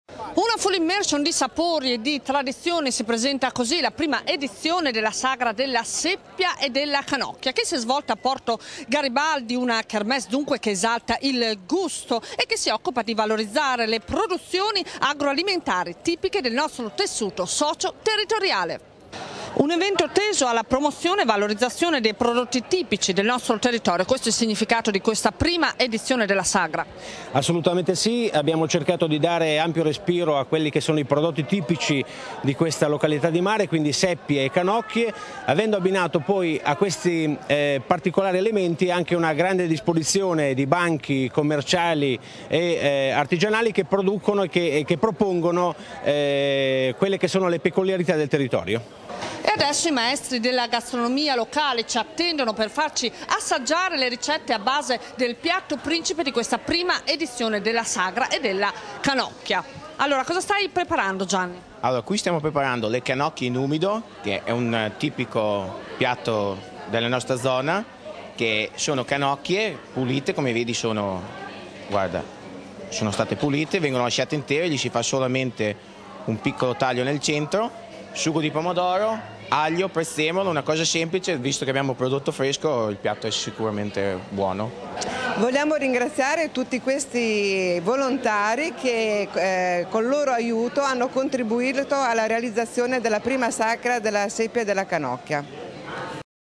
La sagra della seppia
Un incontro ravvicinato con i piatti della tradizione del nostro territorio. Prima edizione della sagra della seppia e della canocchia che si è svolta a Porto Garibaldi. Prodotti tipici abbinati a banchi artigianali e commerciali che producono e che propongono quelle che sono le peculiarità del territorio. Canocchie in umido, lasciate intere, con un piccolo taglio al centro, sugo di pomodoro ed il pasto è servito, sicuramente gustoso dato che a detta degli stessi chef, "il pesce è fresco e sarà sicuramente buono". Con le nostre telecamere siamo andati nel comacchiese per la prima edizione della kermesse gastronomica.